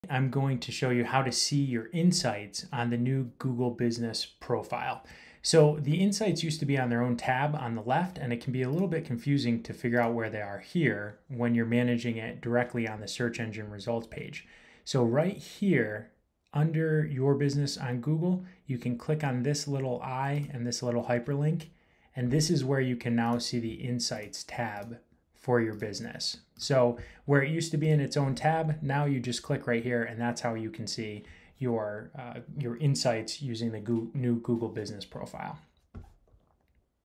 How To Check Insights on New Google Business Profile [ On Maps & Search]
You need to click in a different place to see your business analytics and insights for your Google Business Profile page.

With the old Google My Business profile, you were able to check the insights tab to see information about the performance of your Google Business Profile. Now, with the update to the platform, you need to manage your Google Business Profile on the search engine results page or on maps. To see your insights, you need to click on the hyperlink underneath the words "Your Business On Google".